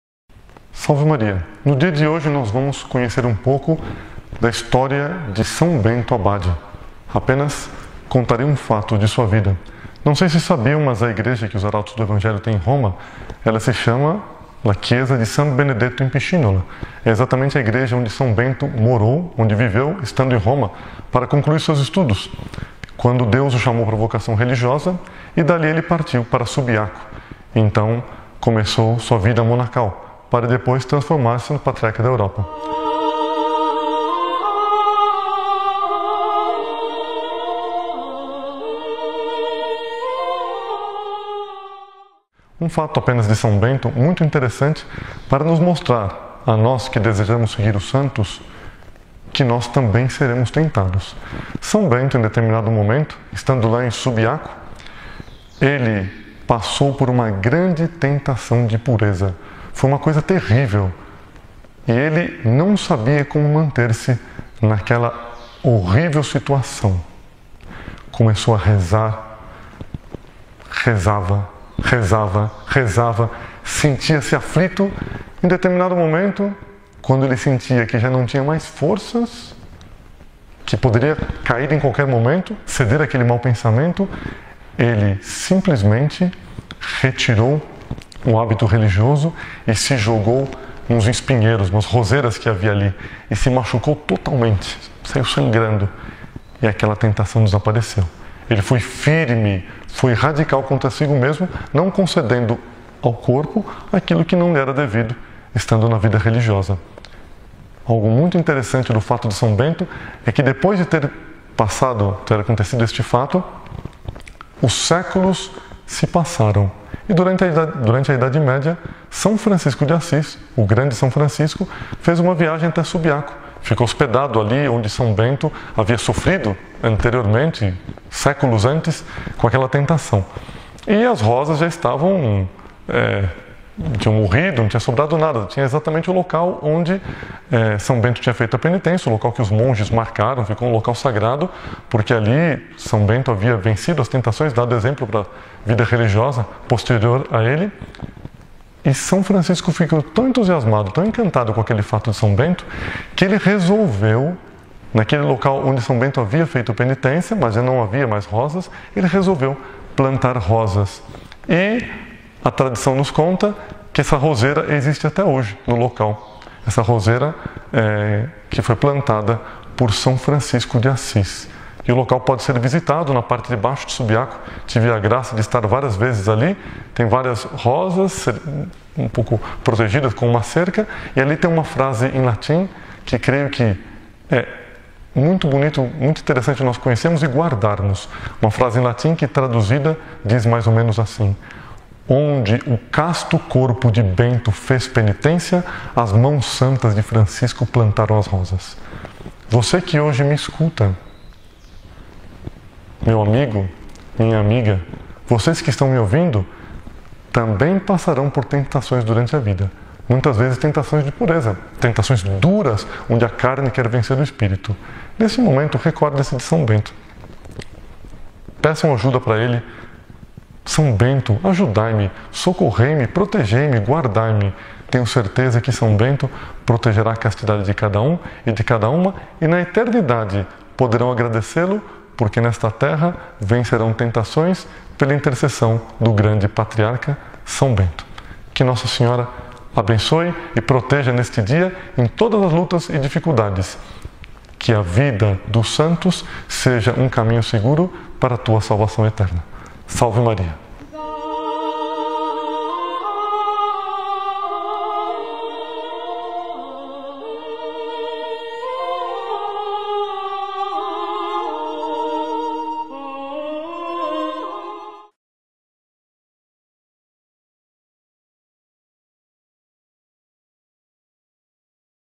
St Benedict and the Rosebush
Walking with the Saints is a series presented by Father Dartagnan de Oliveira, of the Heralds of the Gospel.

This series brings you facts on the lives of the saints, to help during our journey through life.

In today's program, Fr. Dartagnan tells us a story about St. Benedict's temptation against purity and how he conquered it.